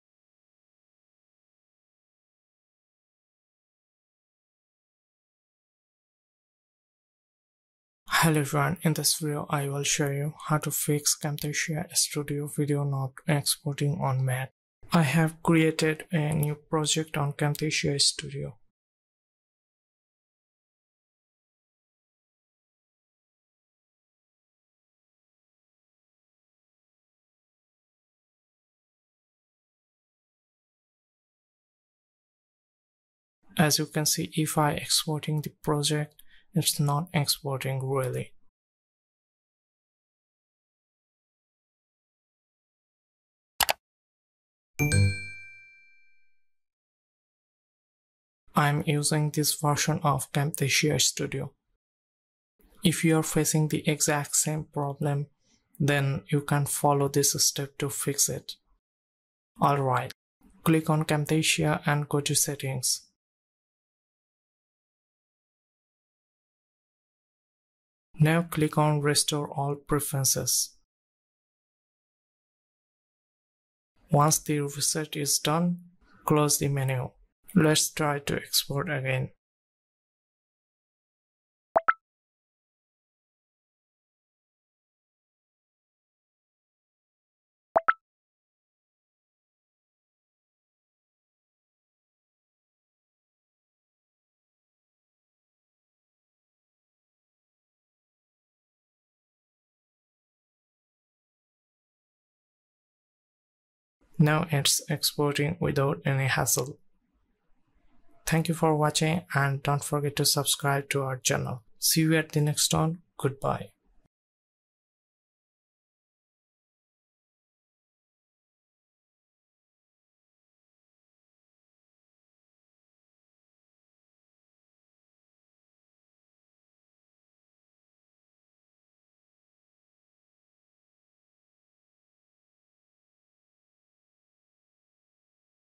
How to Fix Camtasia Studio Video Not Exporting on Mac
𝗦𝗵𝗼𝗿𝘁 𝗗𝗲𝘀𝗰𝗿𝗶𝗽𝘁𝗶𝗼𝗻: When I export, the progress bar appears, hits 100%, and disappears instantly and there's no video exported. Not to my drive, not to google drive either. It's as if it gives up before properly rendering the video.

Q. What version of Camtasia are you currently running?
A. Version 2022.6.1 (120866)

Q.What version of Mac OS are you currently on?
A. Ventura 12.2.1